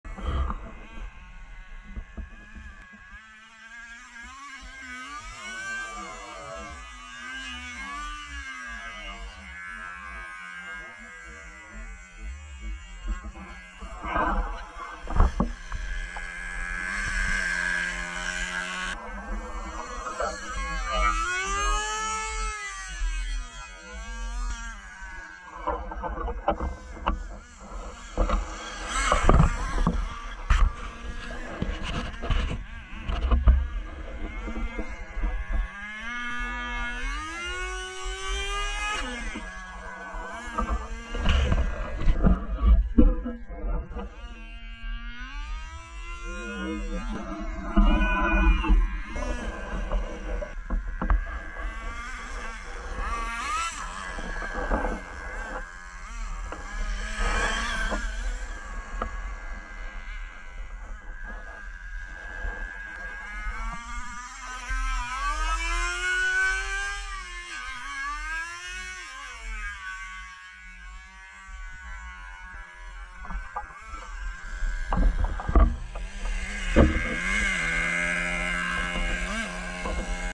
Nitro RC Slow-Mo Jumps
Getting some air-time on my 4WD nitro RC truck. This was filmed in the fall, right after we melted the microwaves with thermite. (One of the remains is being used for the jump)